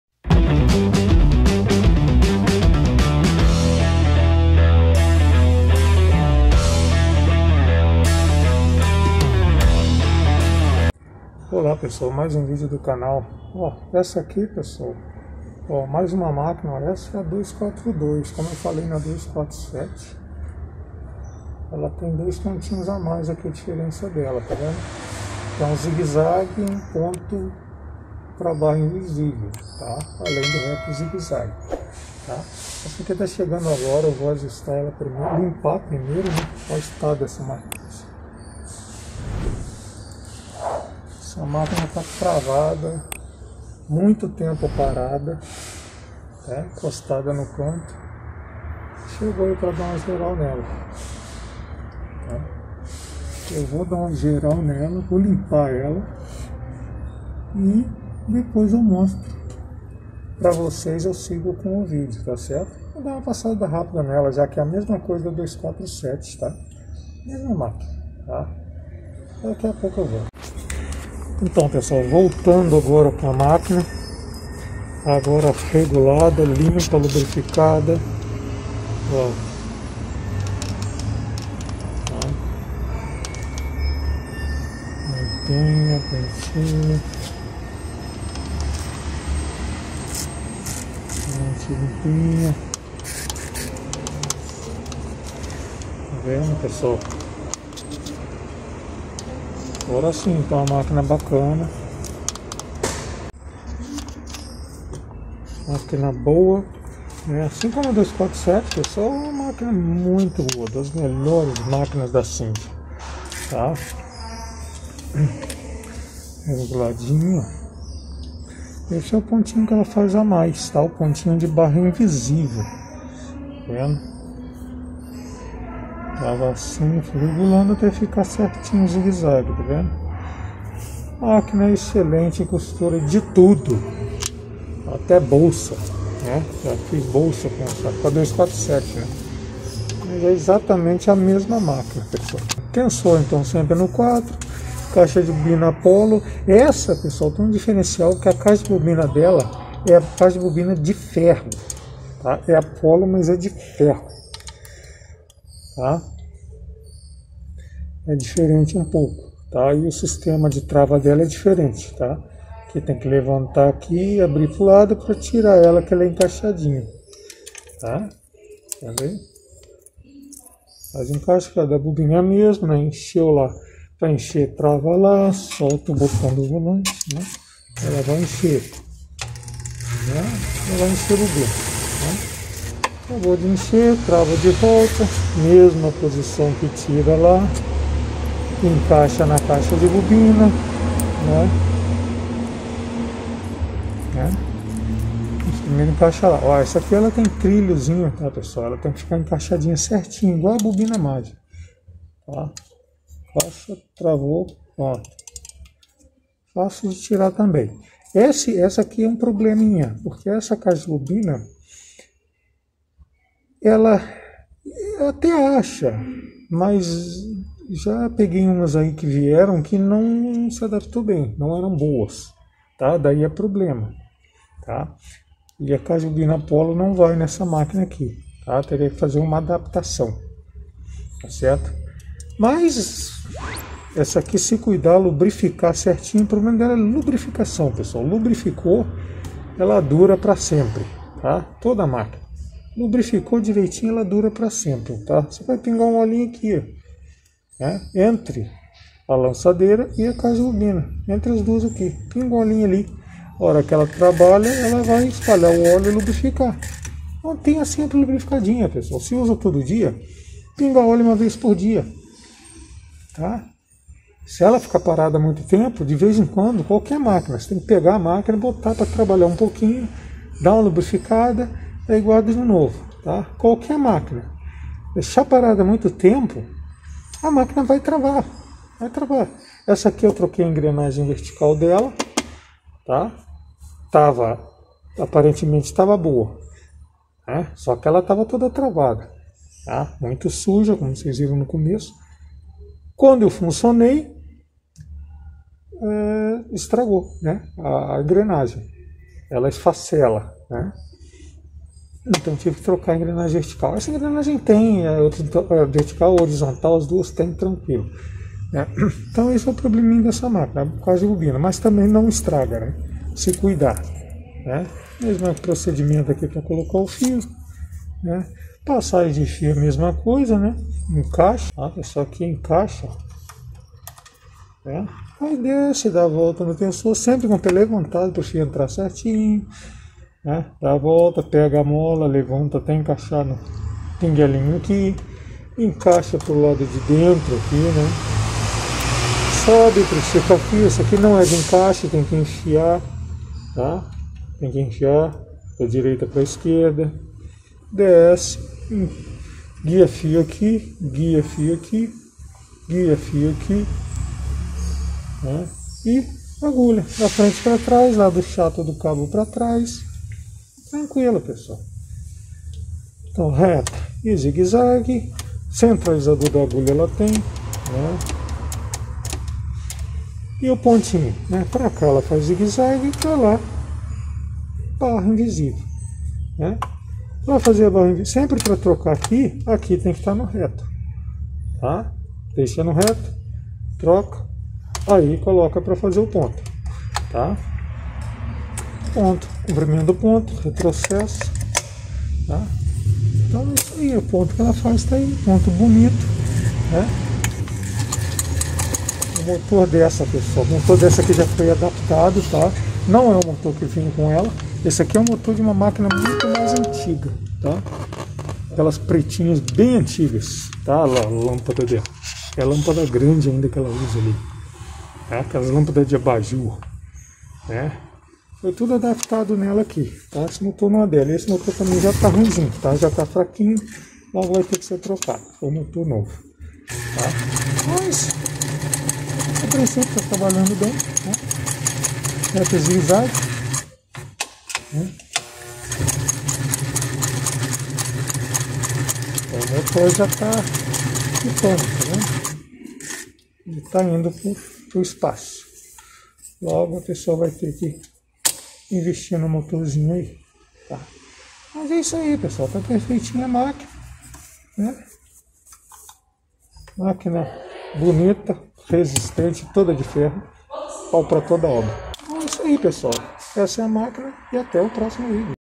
Singer 242 das de ferro!
Olá pessoal.
Segue mais um vídeo agora da 242, da linha das zig zag antigas.
Máquina muito boa!
Similares: 241, 247, 248.